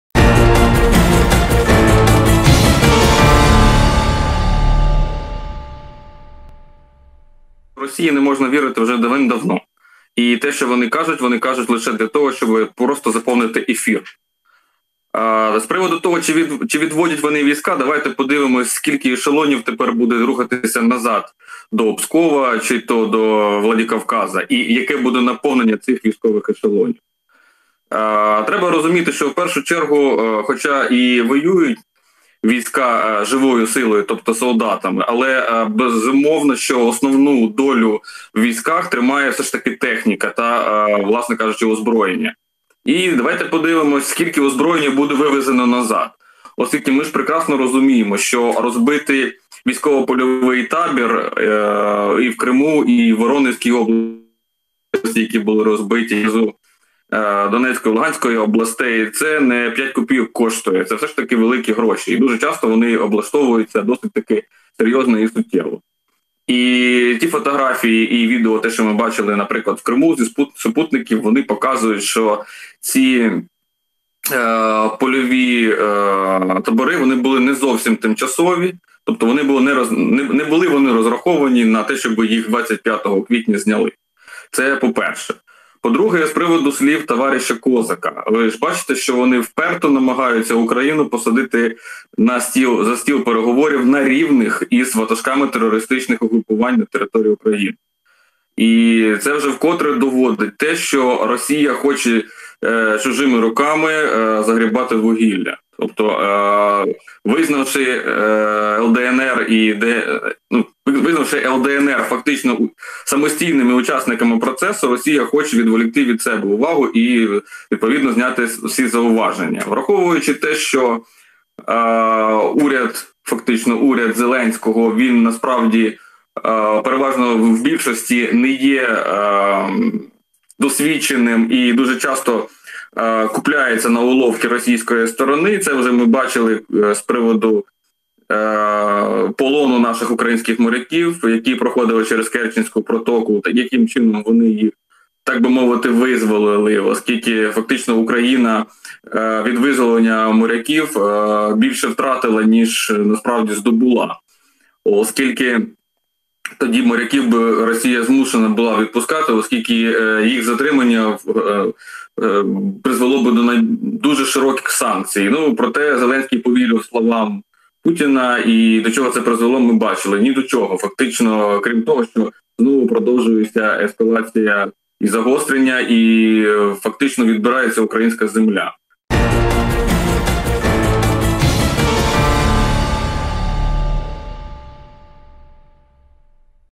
Росія почала відводити війська з кордонів України: коментар експерта
Росія заявила, що починає відводити війська від кордону з Україною, а сьогодні - що війська підуть і з Криму. За словами голови Міноборони РФ Сергія Шойгу, солдати Західного і Південного військових округів з завданнями впоралися та з 23 квітня починають процес повернення в місця постійної дислокації.

Таким чином, за логікою Шойгу, всі стягнуті в останні тижні військові на кордон з Україною повинні будуть піти. Єврокомісія пообіцяла слідкувати за відходом російських військ.

Керівник Офісу Президента України Андрій Єрмак висловив переконання, що відведення російських військ від східних кордонів України є результатом роботи Президента Володимира Зеленського, його команди, а також наших міжнародних партнерів.

Маємо також інформацію, що заступник голови адміністрації президента РФ Дмитро Козак заявив, що Москва запропонувала Німеччині і Франції провести зустріч на Донбасі за участю України і терористичних організацій «Л/ДНР». Представник Кремля встановив дедлайн для зустрічі - 27 квітня
Чи надовго матимемо зниження напруги і хто насправді вплинув на ситуацію ми дізнавалися у Богдана Галенко, політолога.